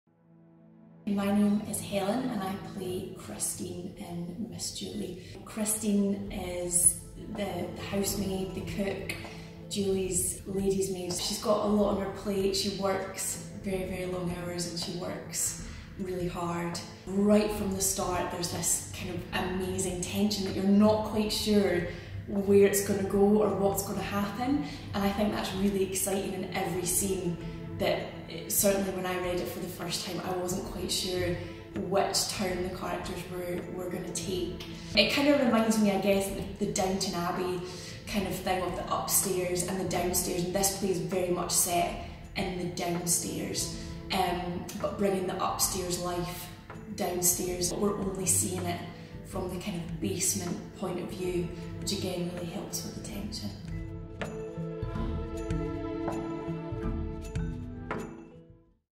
Miss Julie | Helen Mackay
We are loving having Helen back in Perth Theatre. Here she is to tell you what awaits you in Miss Julie - upstairs/downstairs drama, tension and passion.